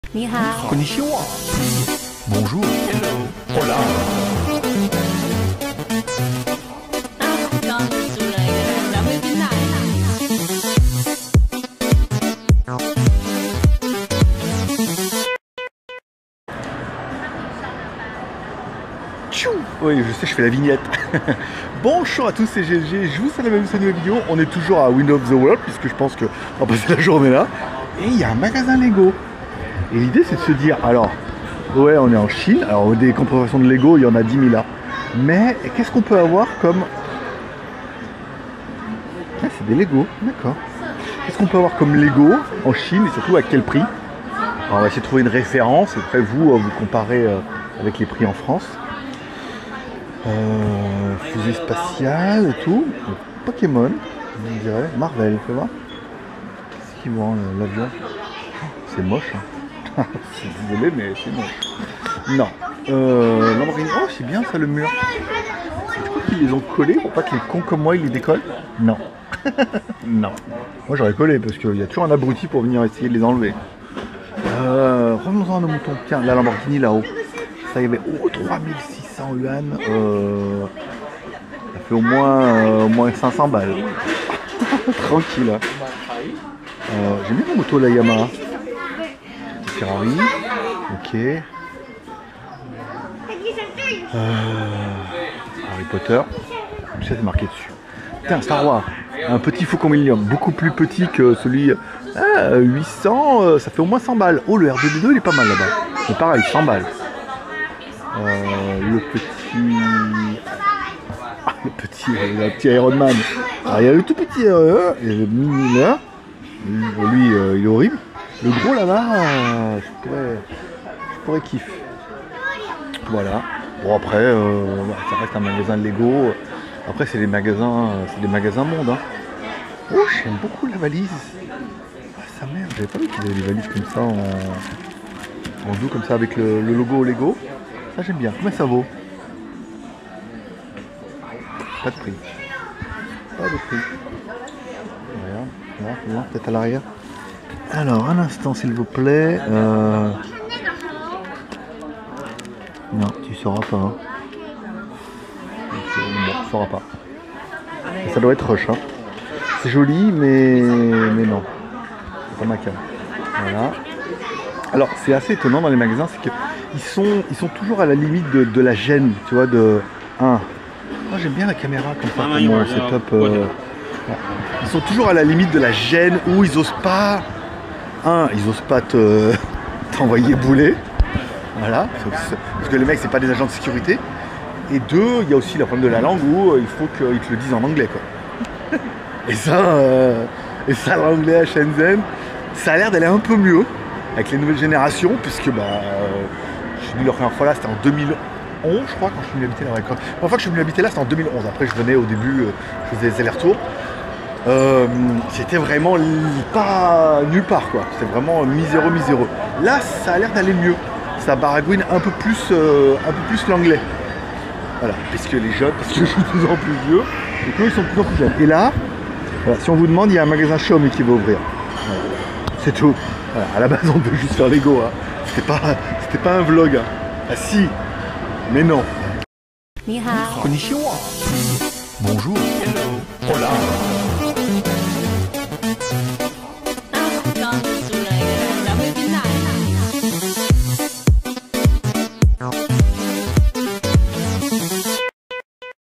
Achetez Vos LEGO à Prix Coûtant au LEGO Store de Shenzhen !
Dans cette vidéo, je vous emmène au LEGO Store de Shenzhen en Chine pour découvrir la gamme complète de LEGO à prix coûtant. Explorez avec moi les meilleures offres et trouvez les sets LEGO qui vous intéressent, tout en profitant de prix incroyables !

By GLG